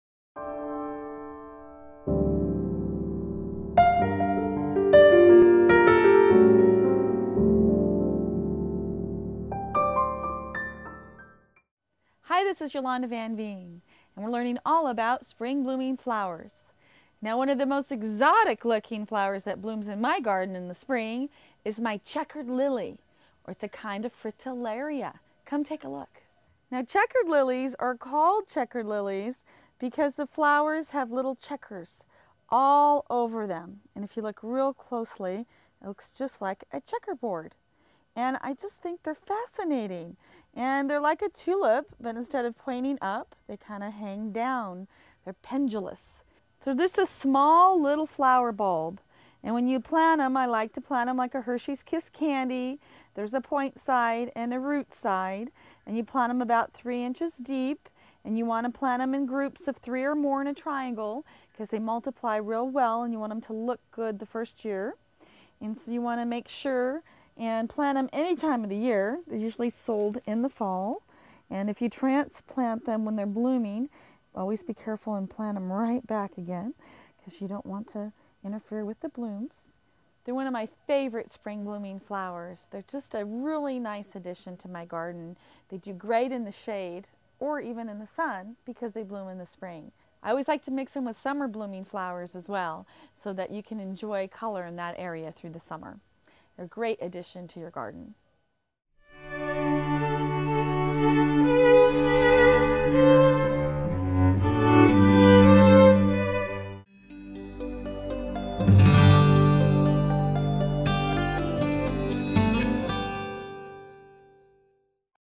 Growing Checkered Lily Fritallaria
Learn how to grow Checkered Lily with Yolanda Vanveen.